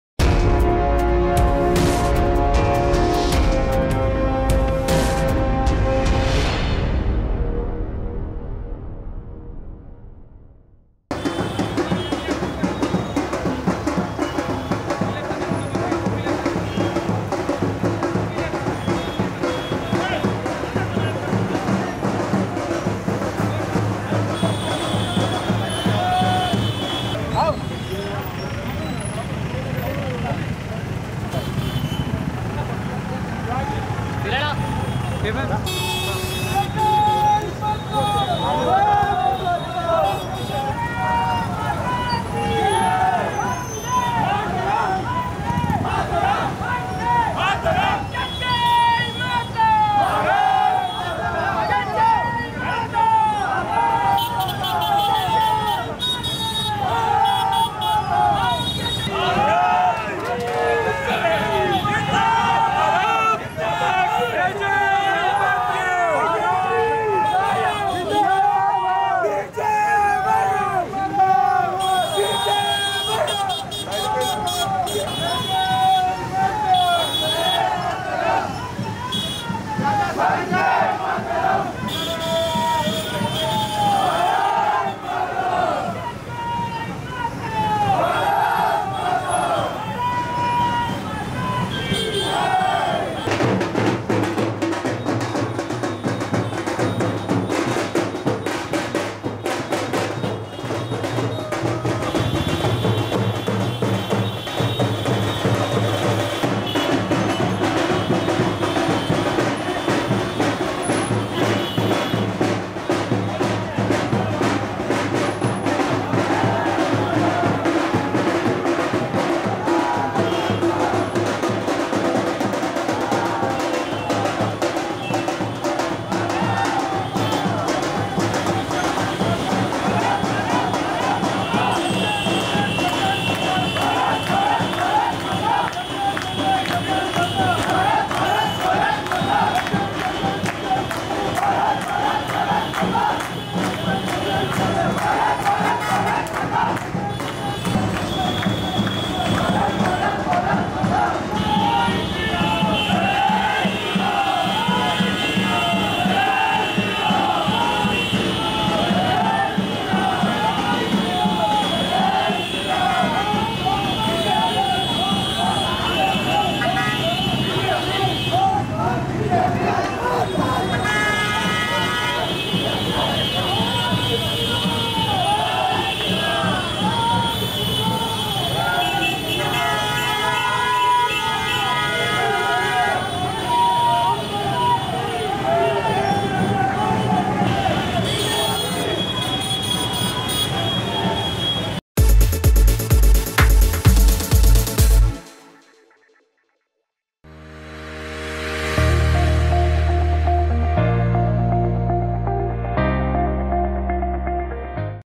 BJP MP Tejasvi Surya Road Show In GHMC Elections 2020 | Hyderabad | CM News
Watch MP Tejasvi Surya Road Show In GHMC Elections 2020 | Hyderabad | BJP | CM News